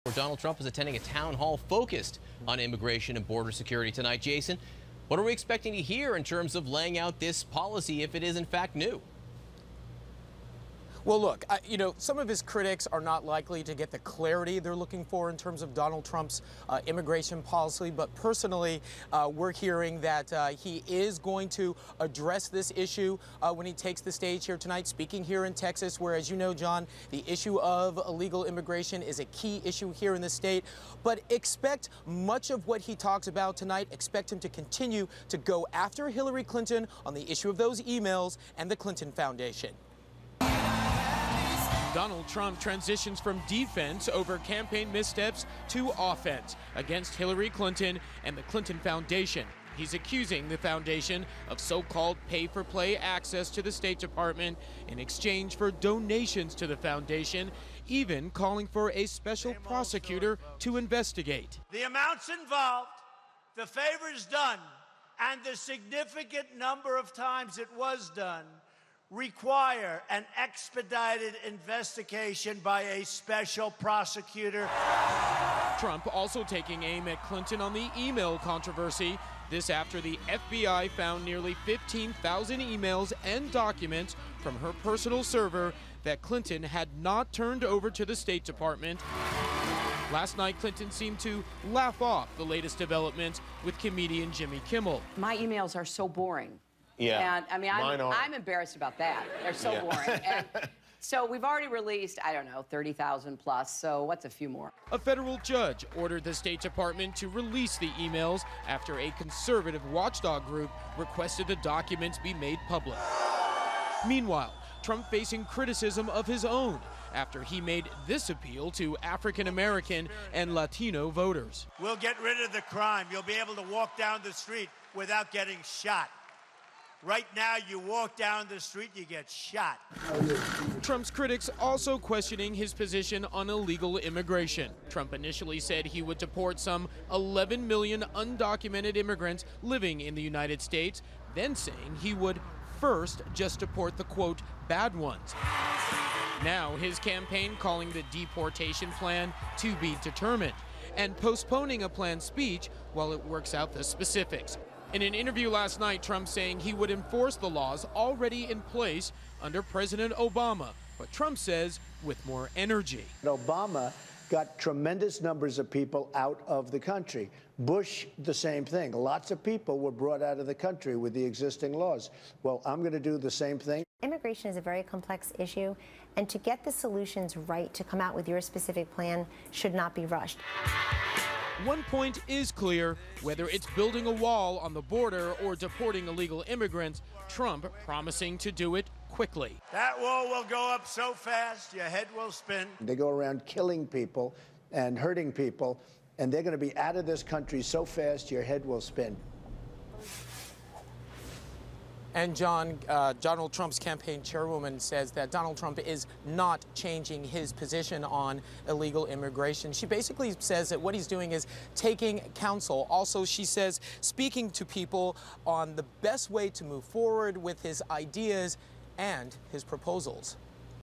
Did Trump just change his immigration plan?
Does Donald Trump still believe in using a deportation force to round up the 11 million undocumented immigrants already in the United States? CNN's Jason Carroll reports.